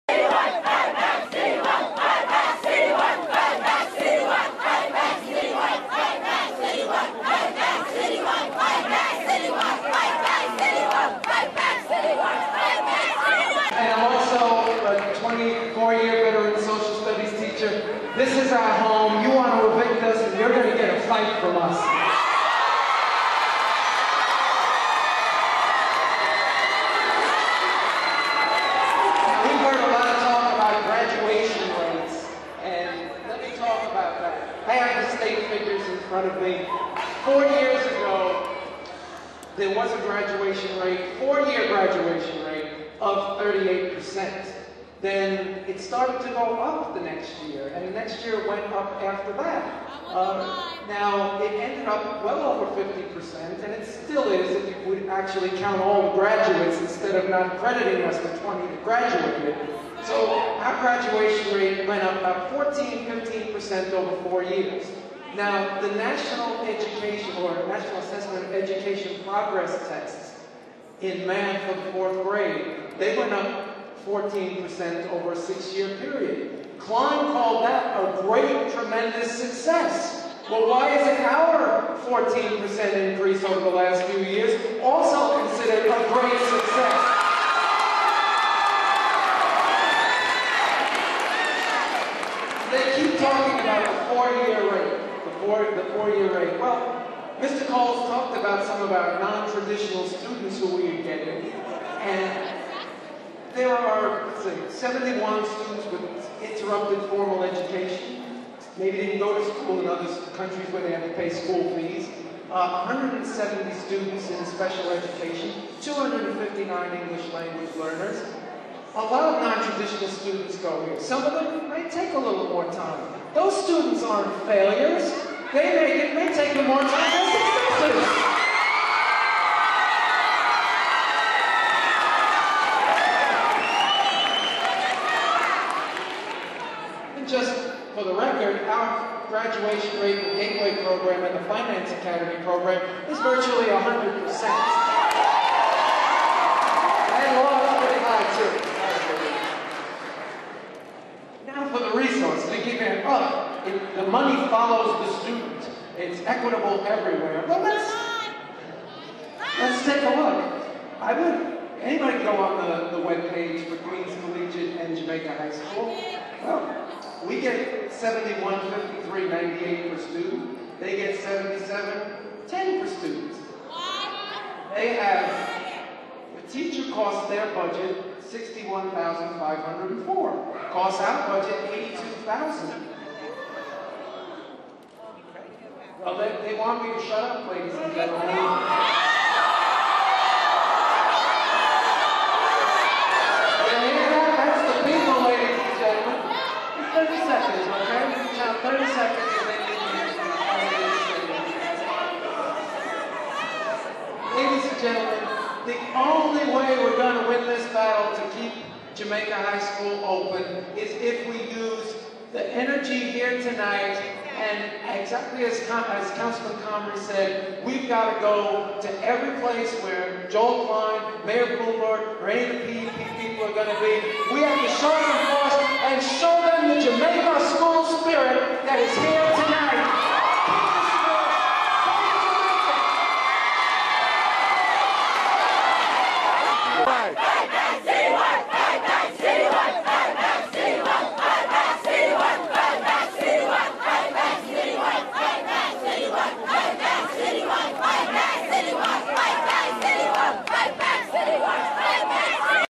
Jamaica High School - James Eterno Against Closure
Dec. 17, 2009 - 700+ Protested Against New York City's Attempt to Shut Down Jamaica High School, Queens.  James Eterno, the UFT (teachers' union) Chapter Leader & member of ICE-UFT speaks to outraged crowd.